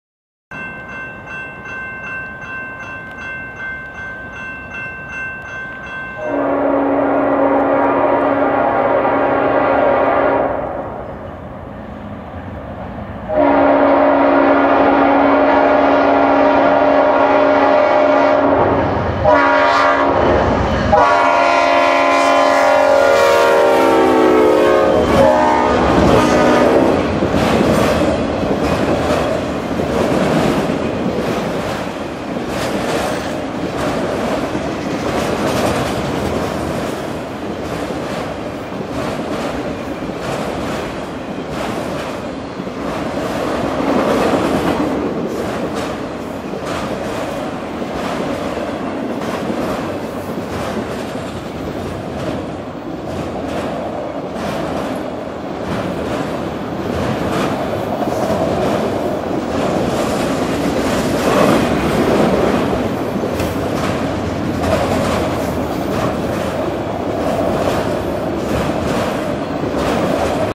Rare: NS Dash-9 9542 has leslie RS5T on 18N at South Plainfield NJ 1/7/12
NS 18N arrives through South Plainfield with its usual Dash 9 duos, but this Dash 9 leader had an interesting and great sounding leslie RS5T horn on it. Not a type of horn you will hear from a Dash 9 everyday..